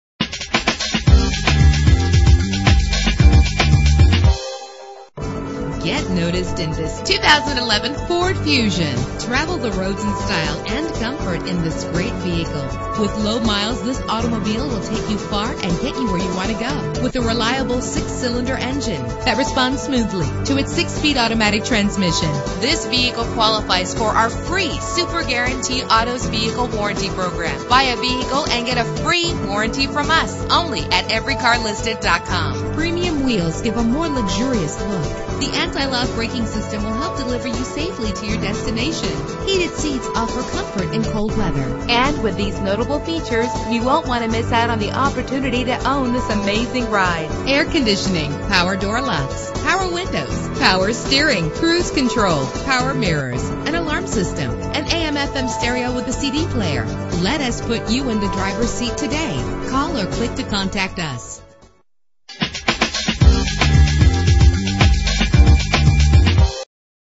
2011 Ford Fusion - Fruitland Park FL
Year: 2011
Make: Ford
Model: Fusion
Trim: SEL
Engine: 3.0 liter 6 cylinder 24 valve
Transmission: 6-Speed Automatic
Color: Silver
Mileage: 9730
Address:
2160 US Hwy 441